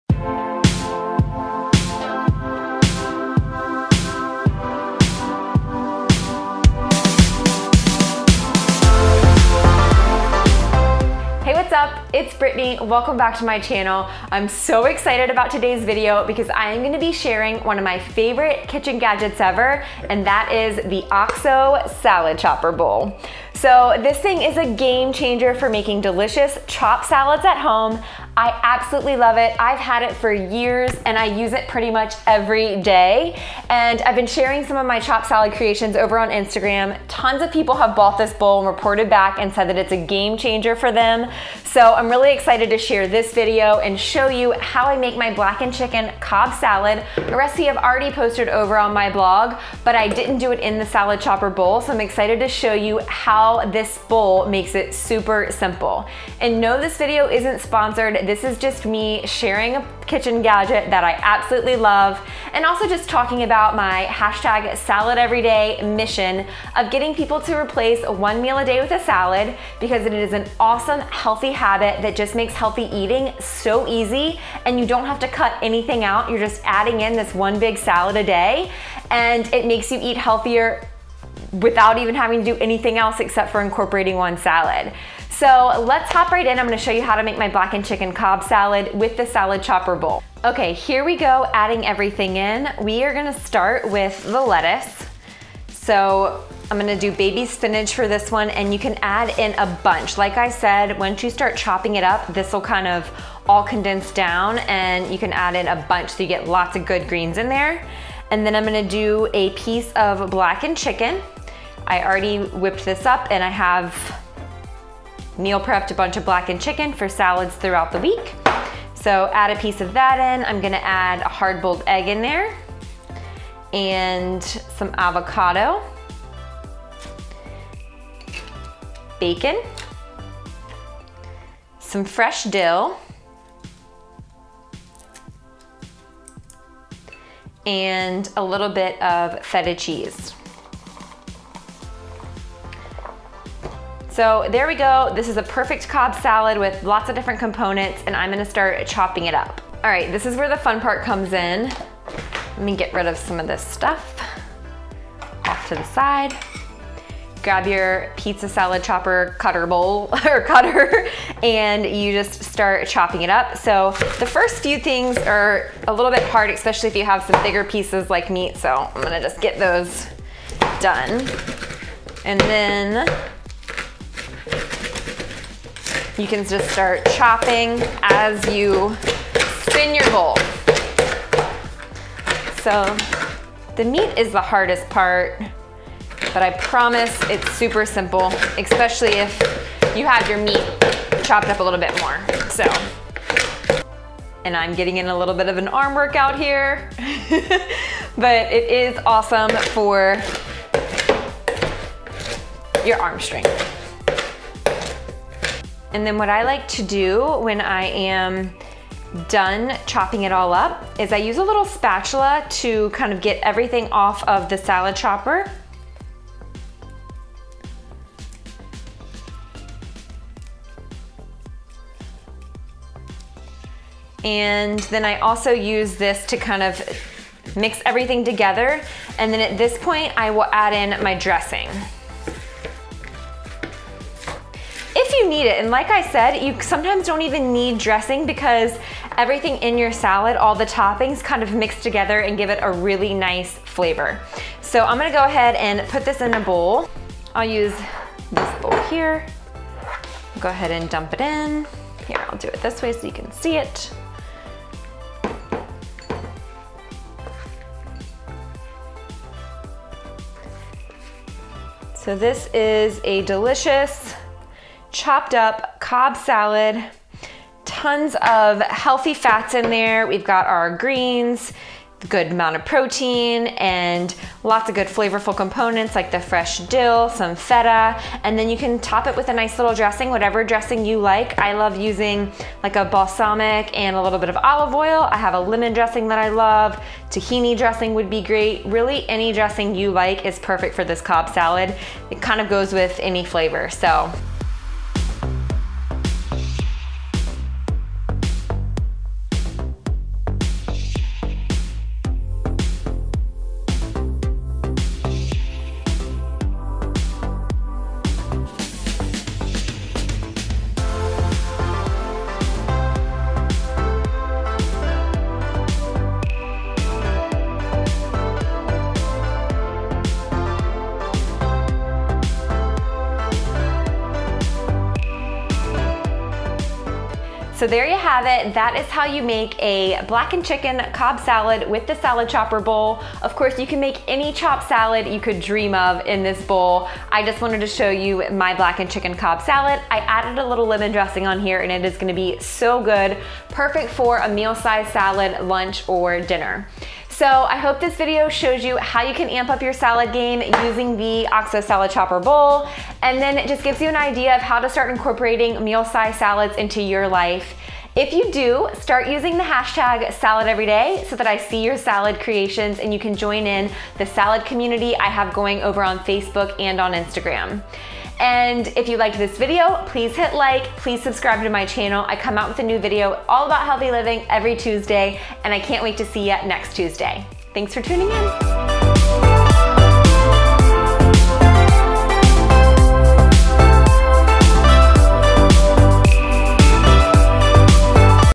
The Salad Chopper Tool You Need For Making Epic Salads
Take your salad game to the next level with the OXO salad chopper bowl. It makes the best chopped salads because everything is chopped into bite-size pieces so you get a taste of each salad ingredient with every bite! Plus, you can toss everything in the bowl and chop it a once... which takes only minutes.